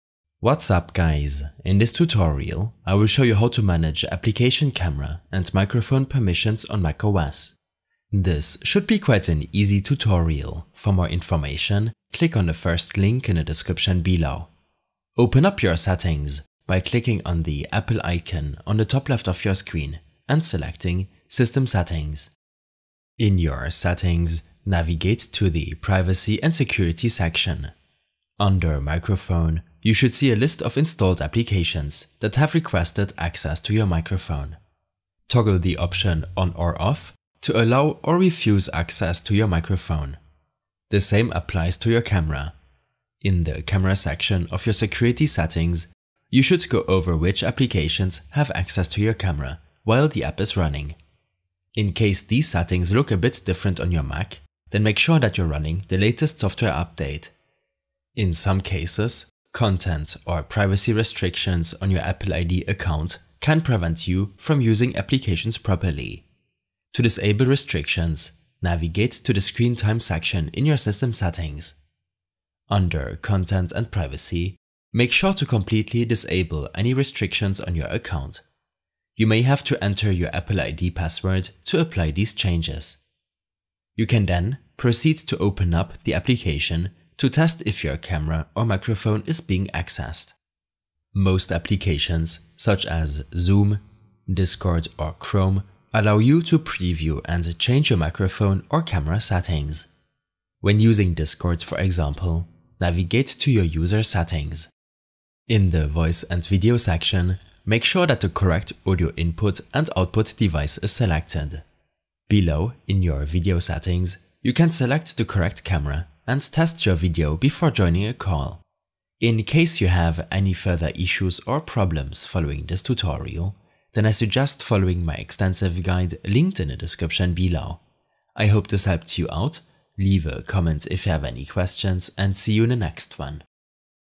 How To Manage Apps Camera & Microphone Permissions on Mac OS
A short tutorial on how to manage application camera and microphone permissions on Mac OS.

Timestamps:
0:00 Introduction
0:14 Privacy & Security Settings
0:28 Allow Access to Microphone
0:40 Allow Access to Camera
0:58 Disable Privacy Restrictions
1:23 Application Cam & Mic Settings